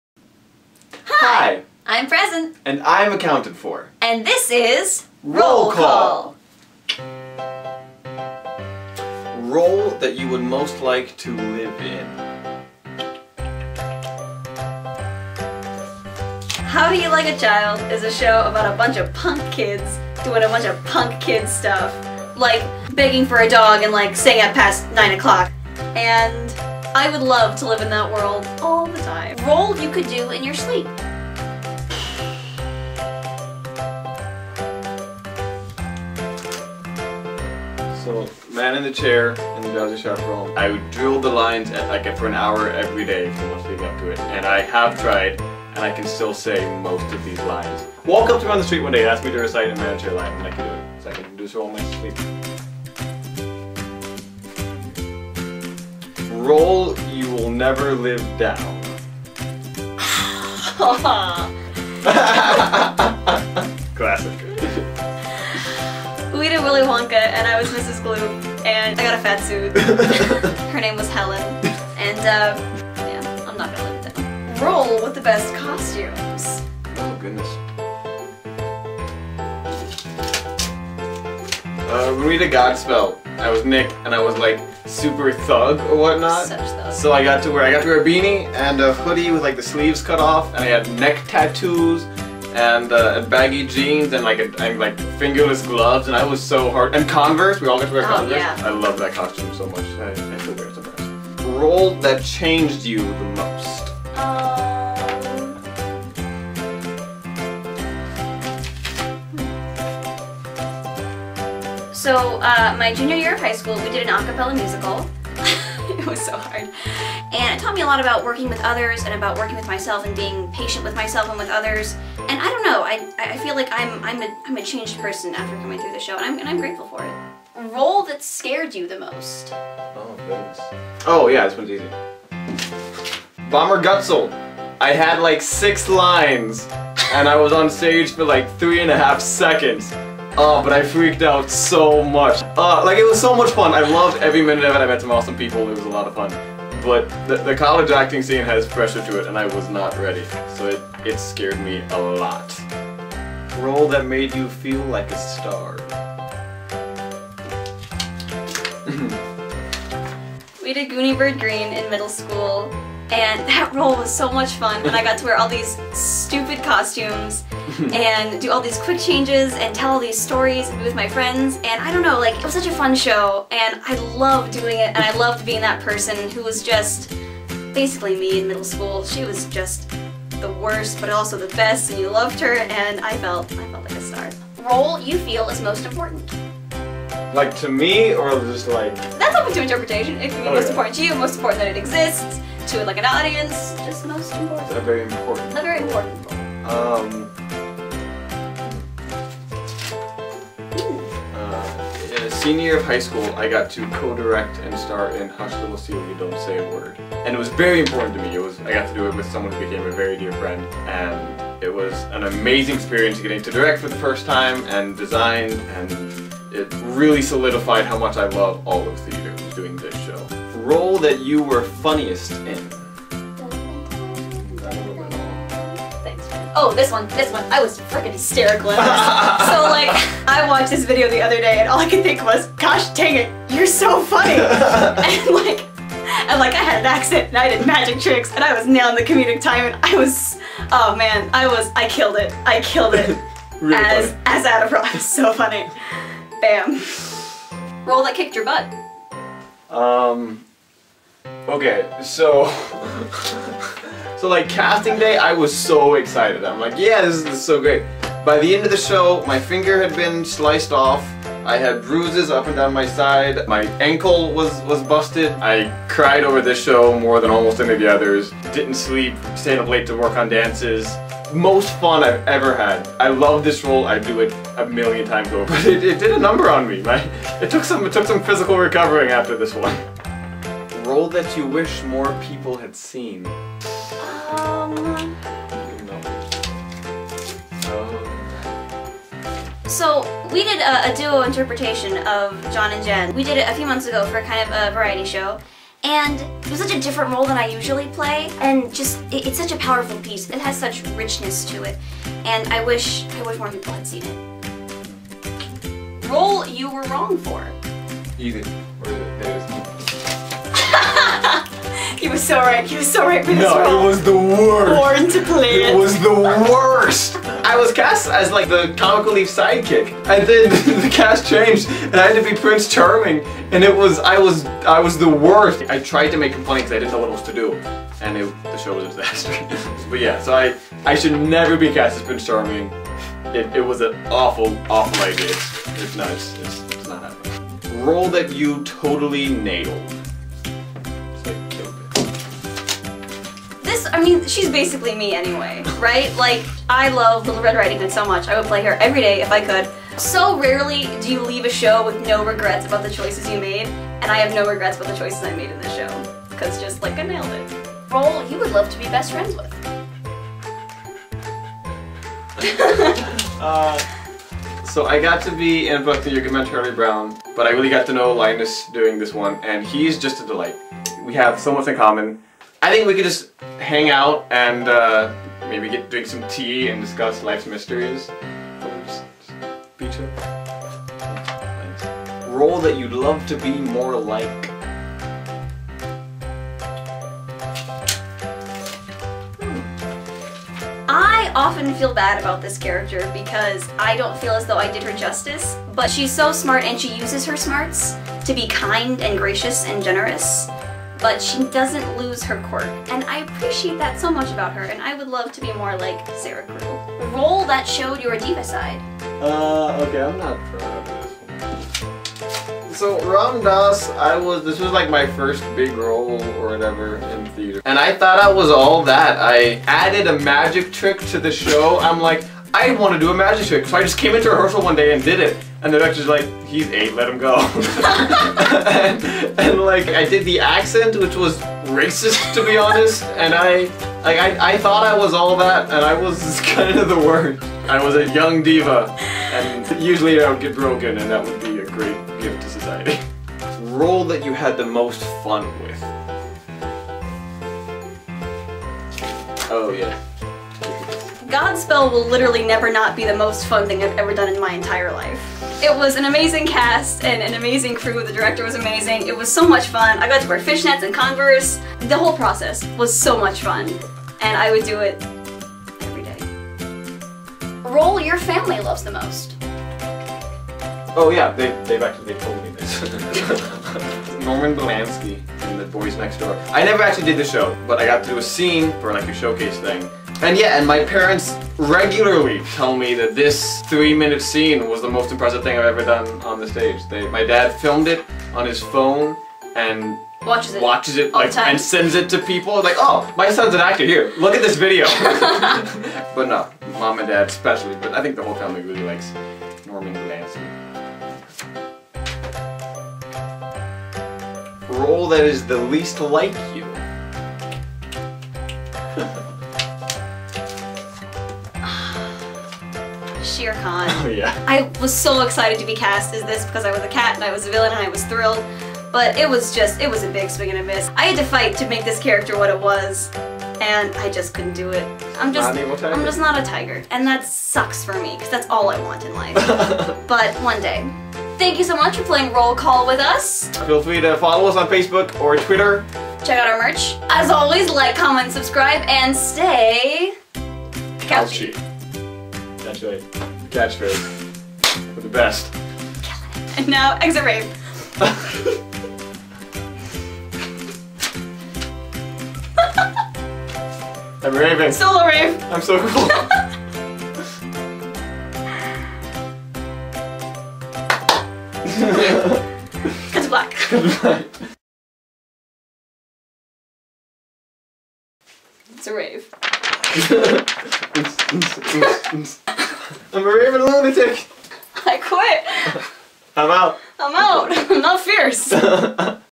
Two Kids On A Couch | Role Call
Weeeeeeeee're baaaaaaack!

We know it's been a long absence, but those two pesky couch-ridden kids are back with another video!  Today, they pay tribute to one of their favorite Broadway.com segments, "Role Call," by examining some of the brightest (and darkest) moments in their theatrical career.

Or Twitter!

We also have merch!